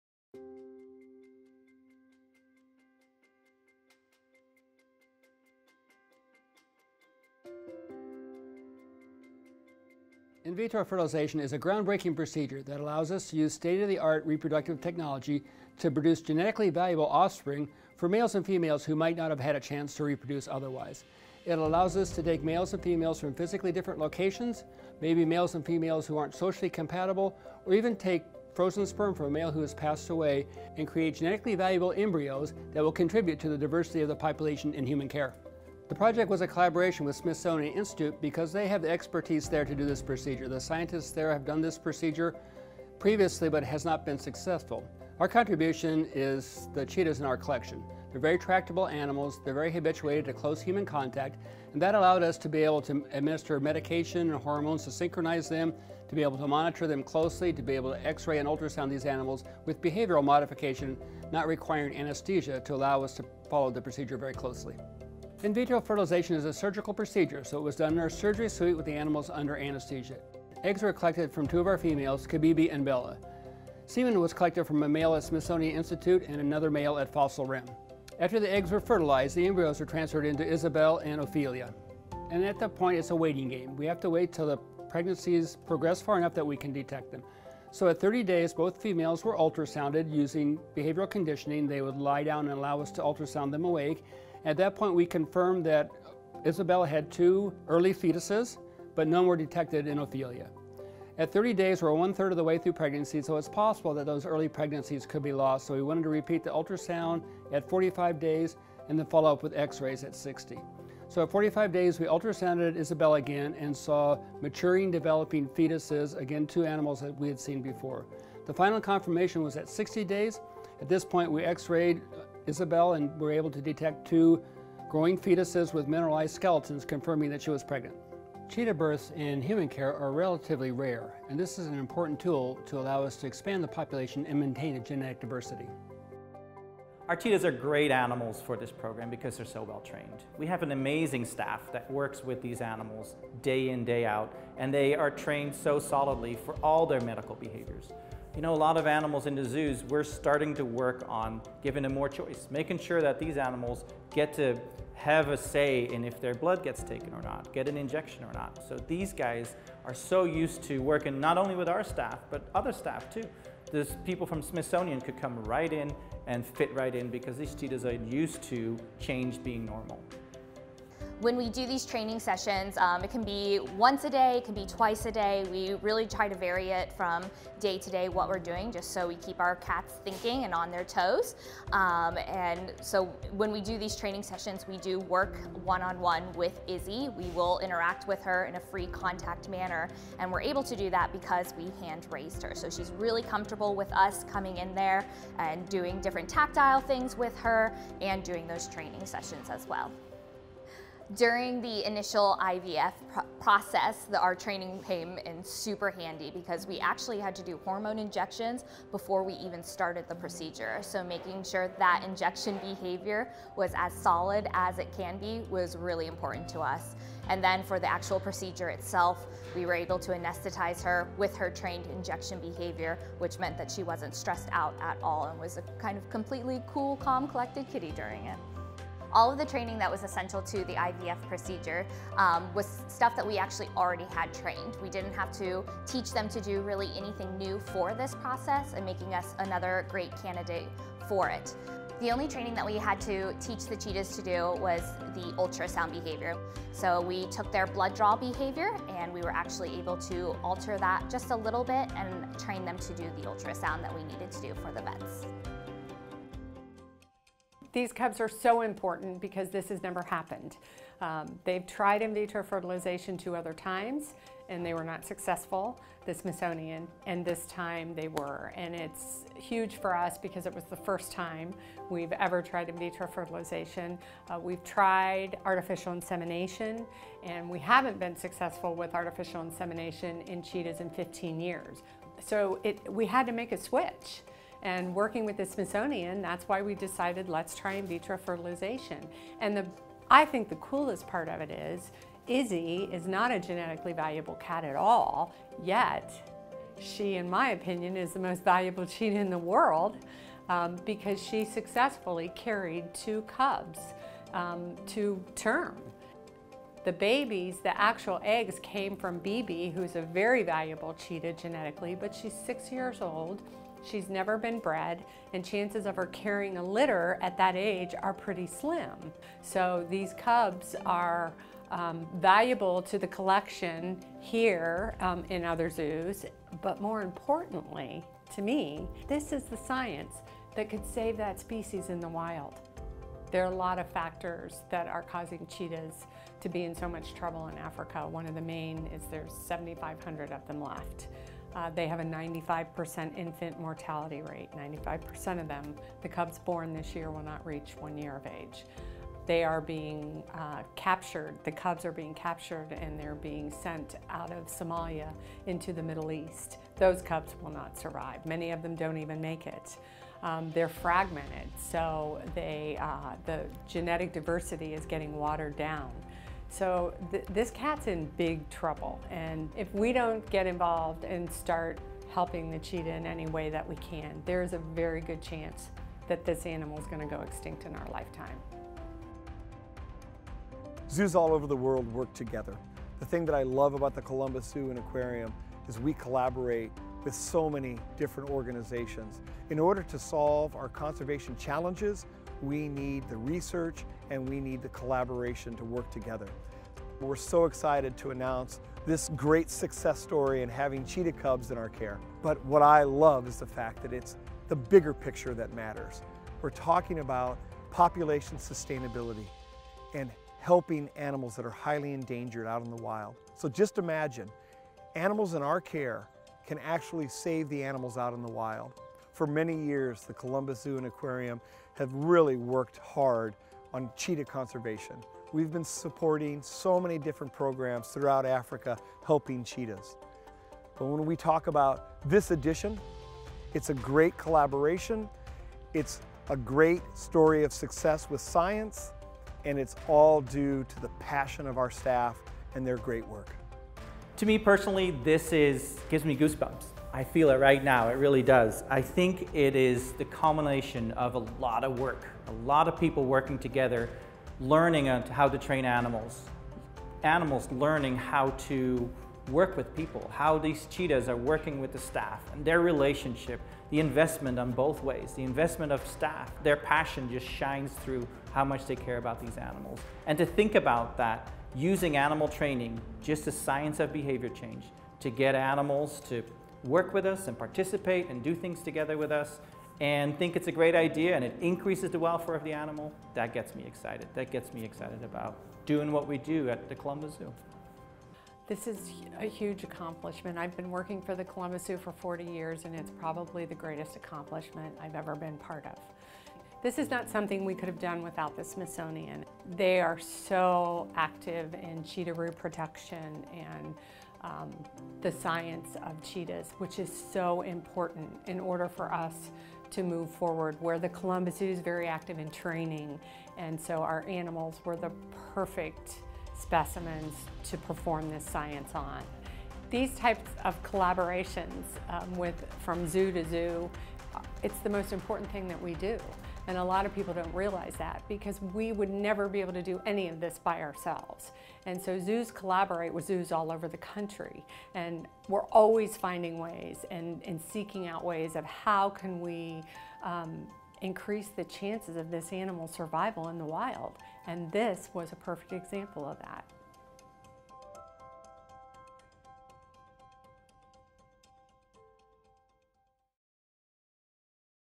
History Making Cheetah Cubs Born Through IVF
History has been made! In a groundbreaking scientific breakthrough, two cheetah cubs have been born through in vitro fertilization and embryo transfer into a surrogate mother at the Columbus Zoo and Aquarium. These efforts were also a part of a breeding recommendation from the Association of Zoos and Aquariums’ (AZA) Species Survival Plan® (SSP) and the Cheetah Sustainability Program (CSP), developed to manage a sustainable population of cheetahs in human care. Watch and learn more about what went into making this a success!